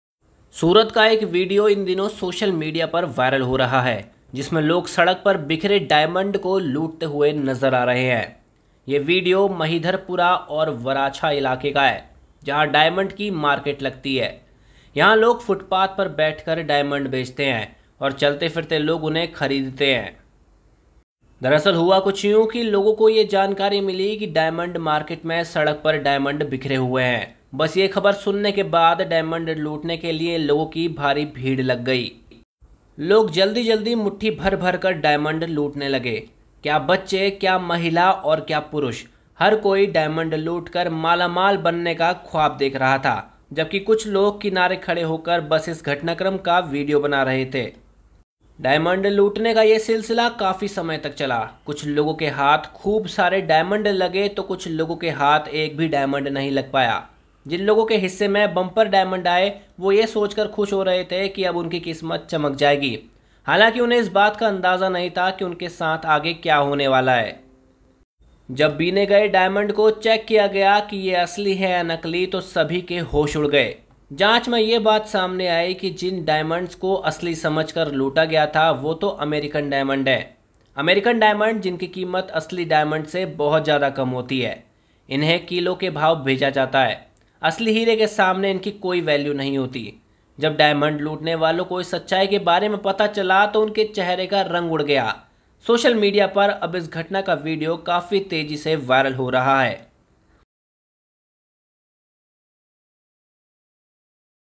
सड़क पर 'हीरे' देखकर लोगों के उड़े होश! लोग मुट्ठी भर-भरकर डायमंड लूटने लगे | Surat Diamonds
सड़क पर 'हीरे' देखकर लोगों के उड़े होश! लोग मुट्ठी भर-भरकर डायमंड लूटने लगे | Viral Video | Surat Diamonds